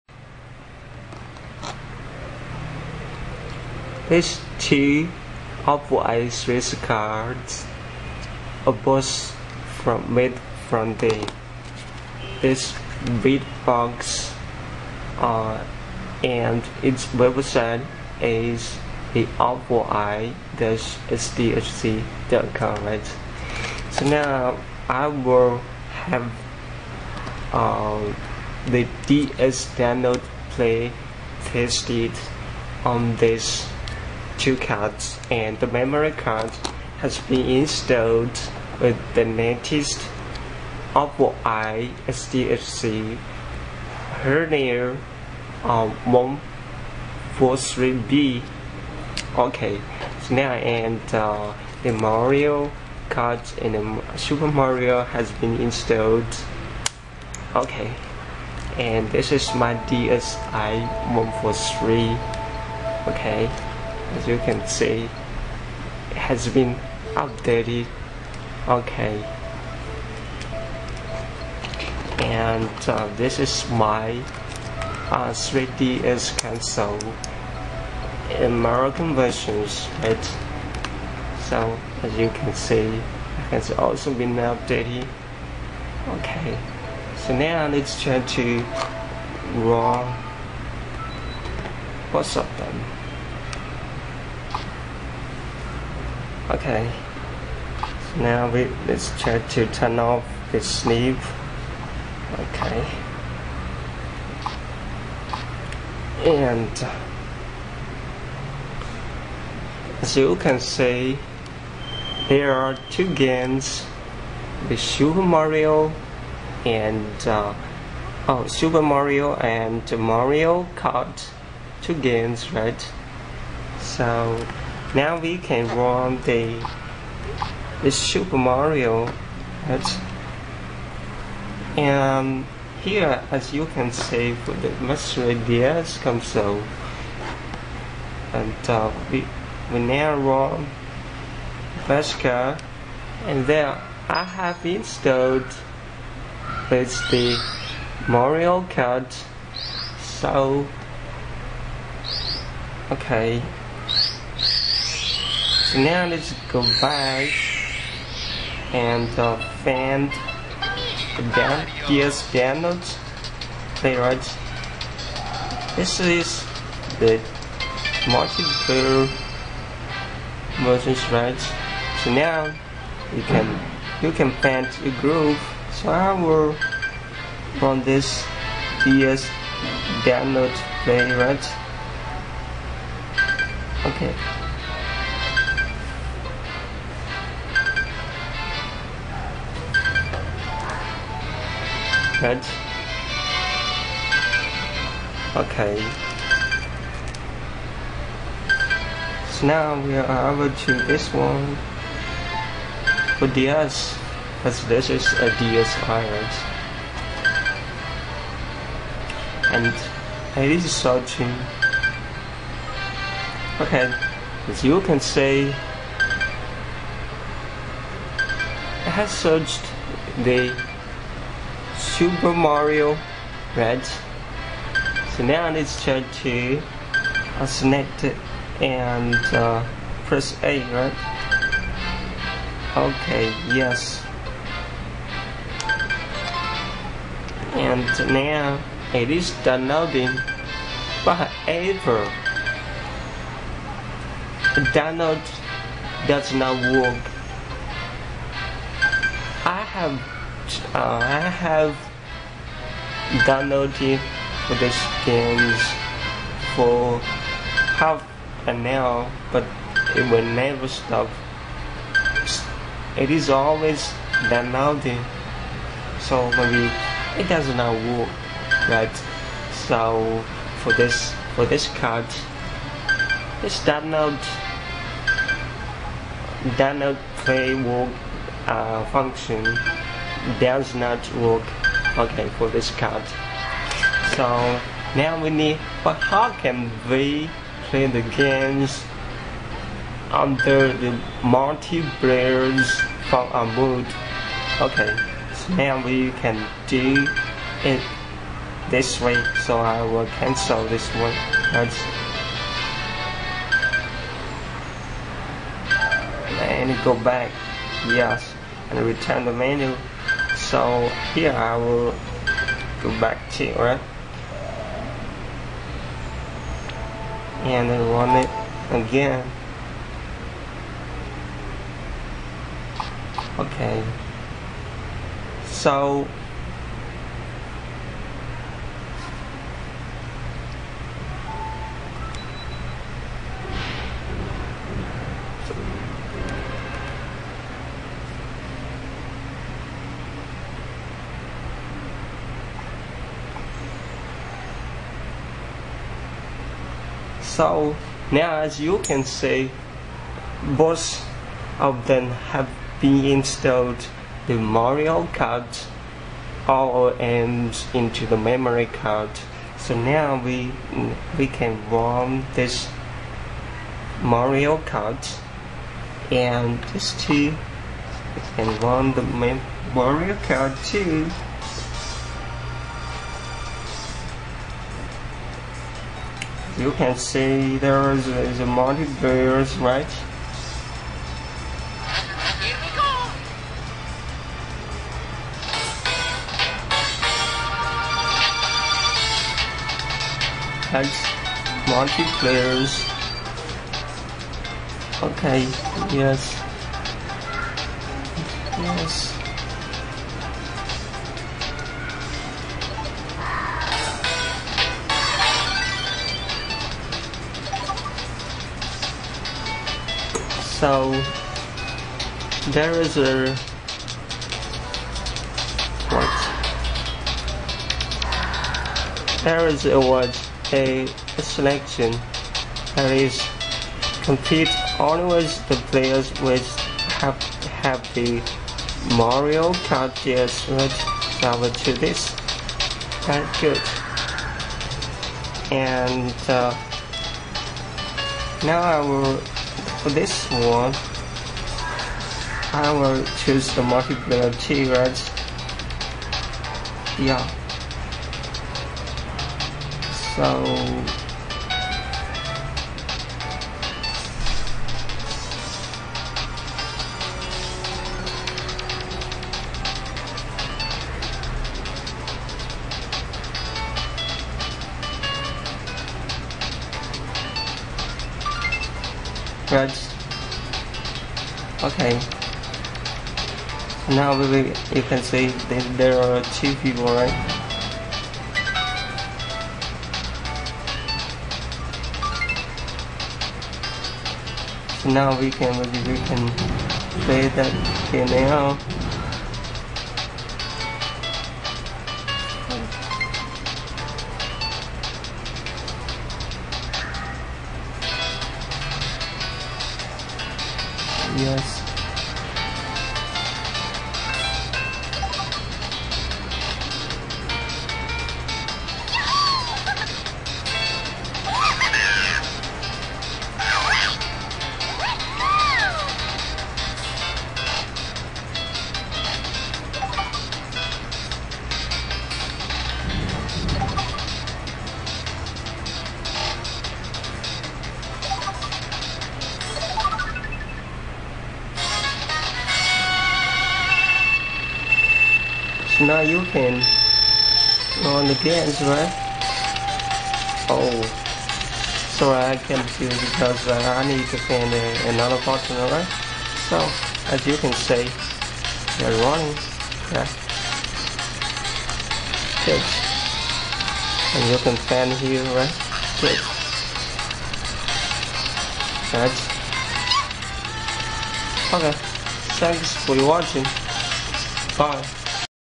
R4i SDHC 3DS Card DS Download Play Tutorial.flv
R4I SDHC card, could not run DS Download Play Function successfully. But users can still play under mutiplay mode if users save the same game ROMs into their memory cards.

They are tested on DSi 1.43J and 3DS V2.1.0-4U.